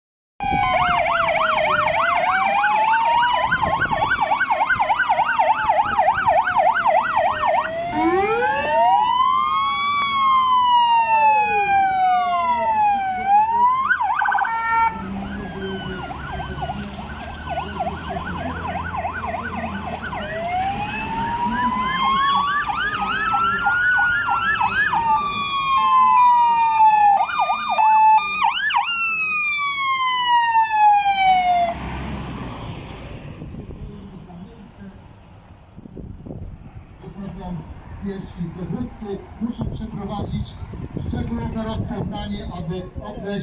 Alarmowo zastępy na pokaz podczas IFRE-EXPO
Alarmowo zastępy z JRG 3 i 1 na pokazy podczas IFRE-EXPO 2019